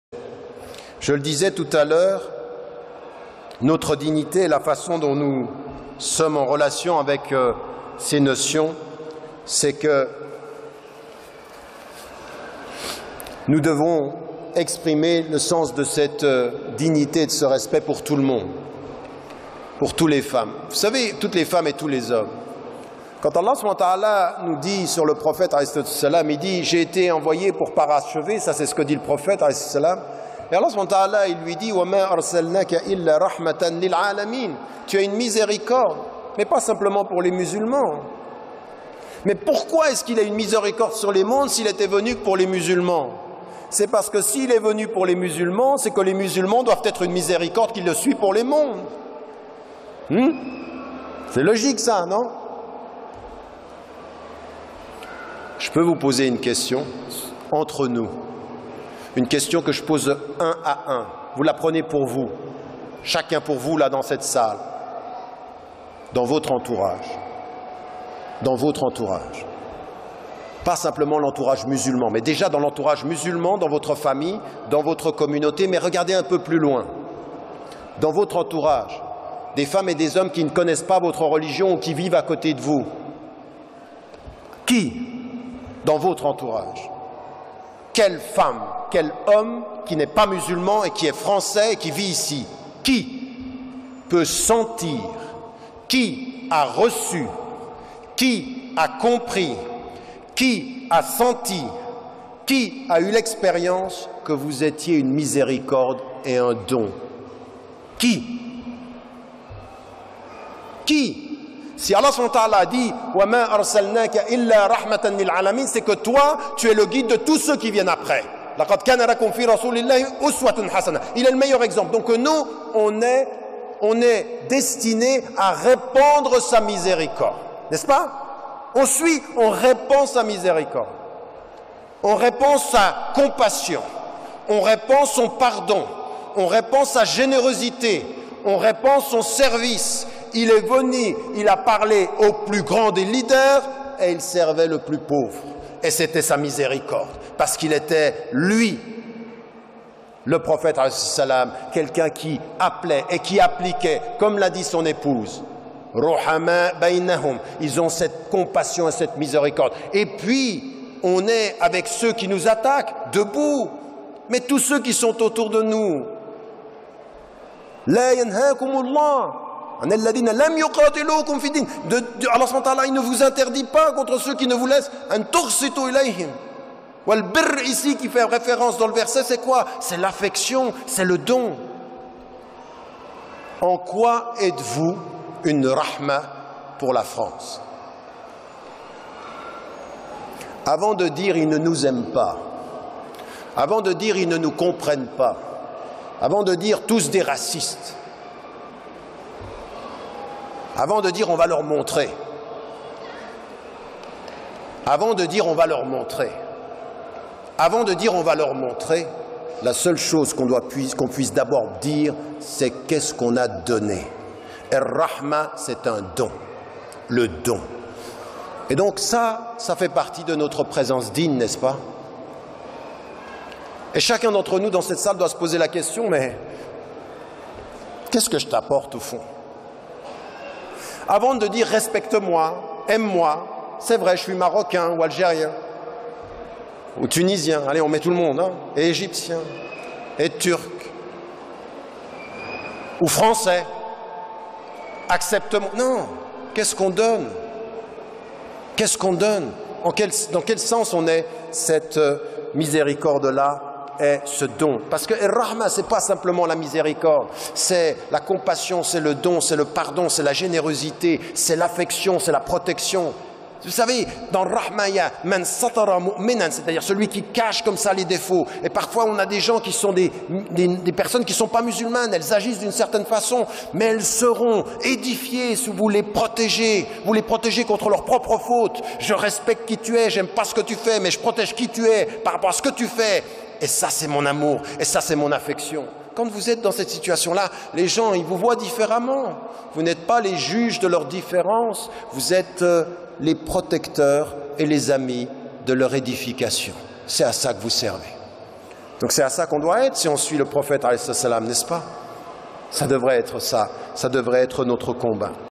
Tariq Ramadan - Mais dites moi en quoi êtes-vous une Miséricorde pour la France ?!
Dr. Tariq Ramadan  Bourget 2013